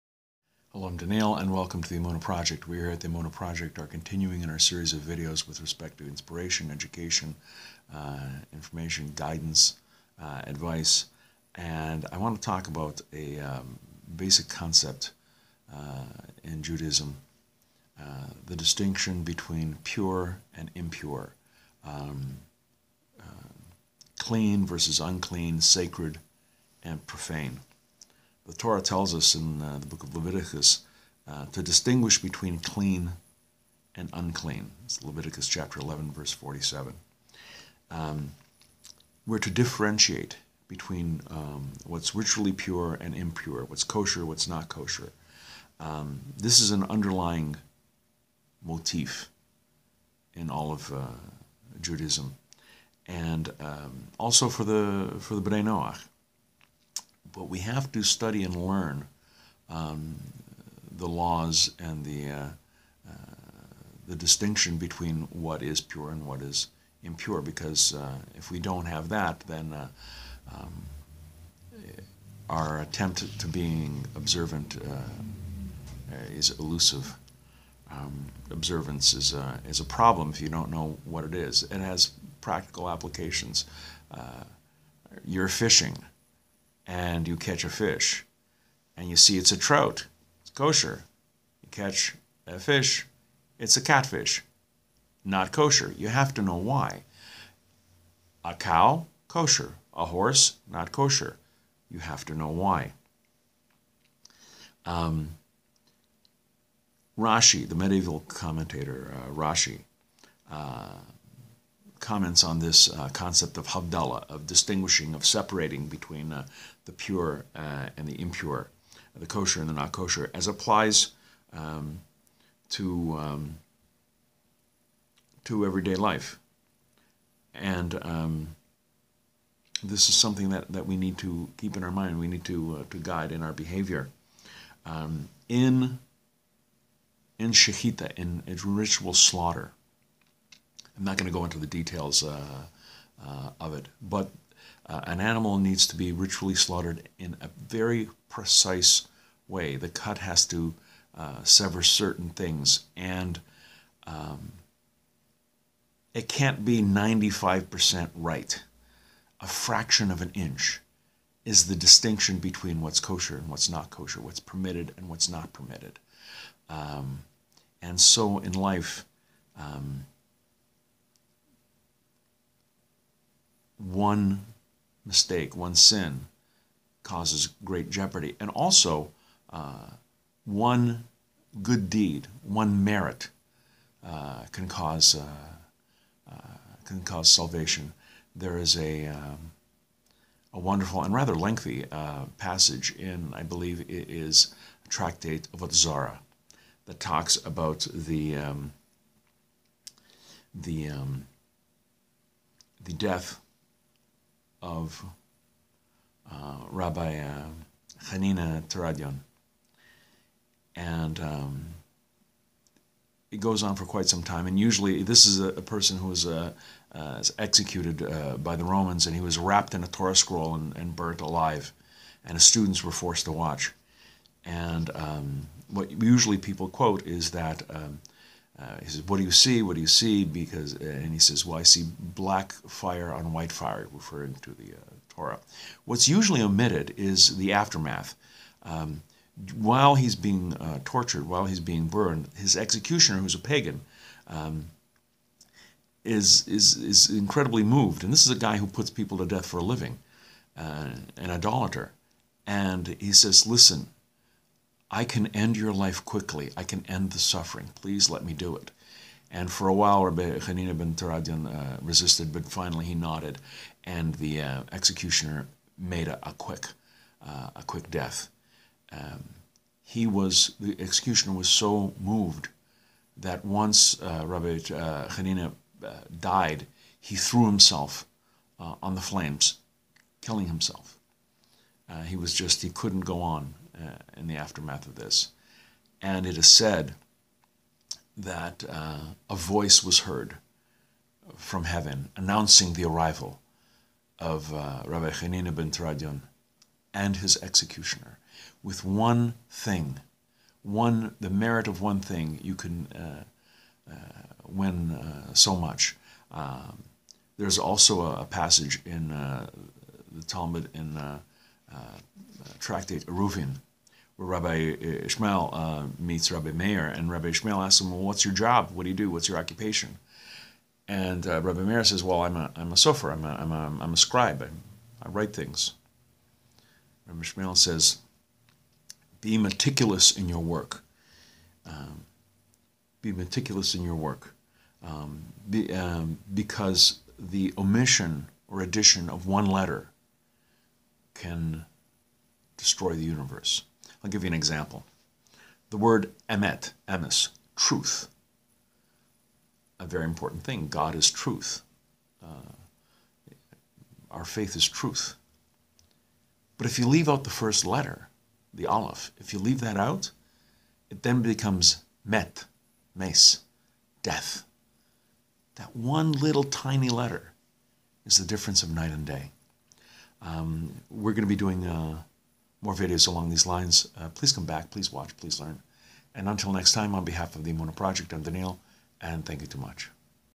Basic Concepts - 22 - Pure vs Impure
The Emunah Project. Pure vs Impure. Permitted vs Forbidden. Kosher vs Non-kosher. Issur v'Heter. Leviticus 11:47.